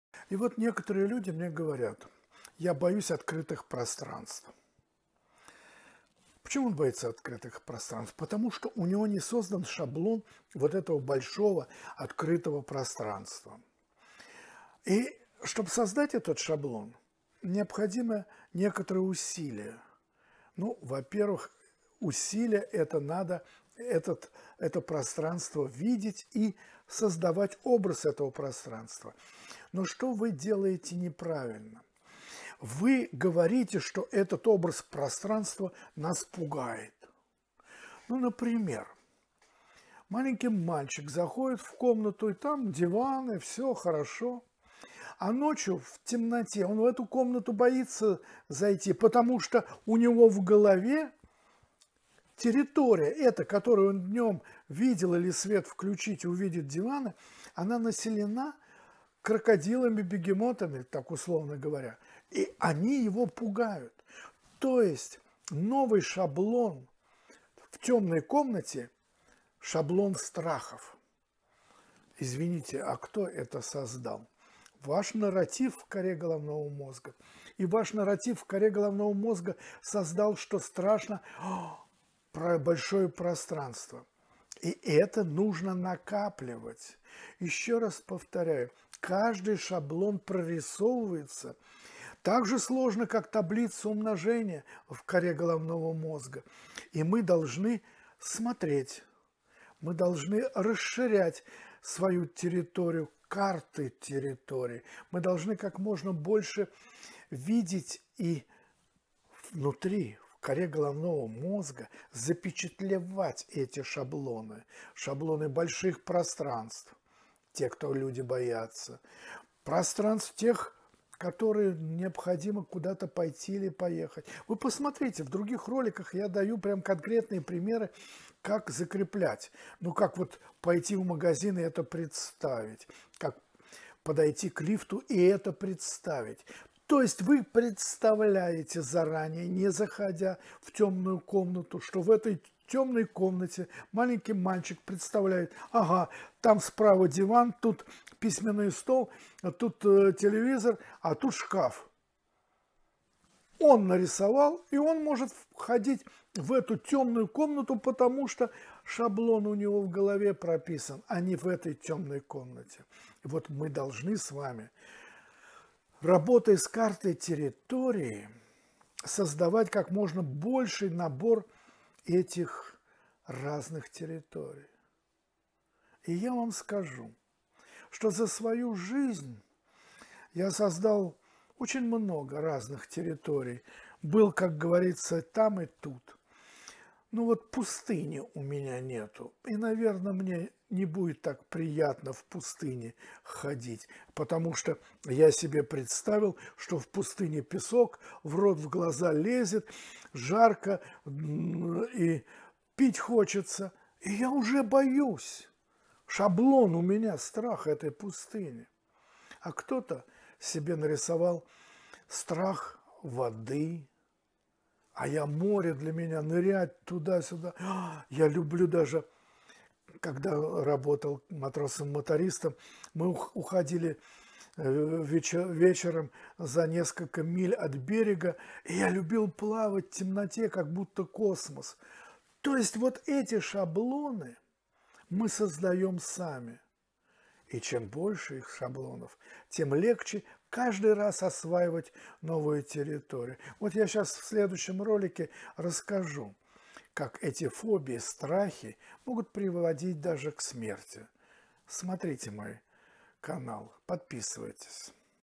избавляемся от фобий пространств агорафобия и социум изоляция
Для жизненного пространства человека важное значение имеет взаимодействие с территорией.
С тем пространством в котором живет человек. Значение Карты Территории трудно переоценить для человека.
МОИ КОНТАКТЫ
Skype: yristreamlet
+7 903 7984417

канал практикующего психолога консультации семьи и невротиков, психосоматические заболевания,